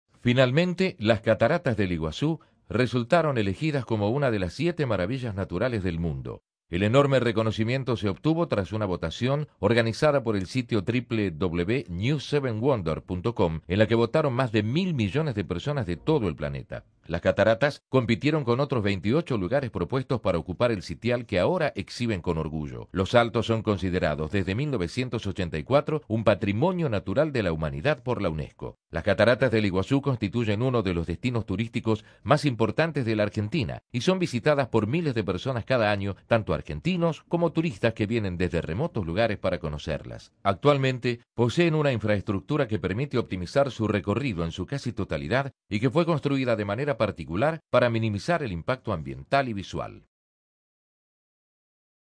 Las Cataratas del Iguazú resultaron elegidas como una de las 7 maravillas naturales del mundo
El enorme reconocimiento se obtuvo tras una votación organizada por el sitio en la que votaron más de mil millones de personas de todo el planeta.
Los resultados preliminares ubican a las Cataratas del Iguazú junto a otras 6 maravillas entre las que figuran la Amazonia y el Dragón de Komodo.
Los saltos son considerados desde 1984 un Patrimonio Natural de la Humanidad por la Unesco y  compitieron con otros 28 lugares propuestos para ocupar el sitial que ahora exhiben con orgullo.
Las Cataratas del Iguazú constituyen uno de los destinos turísticos más importantes de la argentina y son visitadas por miles de personas cada año, tanto argentinos como turistas que vienen desde remotos lugares para conocerlas.
Actualmente poseen una infraestructura que permite optimizar su recorrido en su casi totalidad y que fue construida de manera particular para minimizar el impacto ambiental y visual.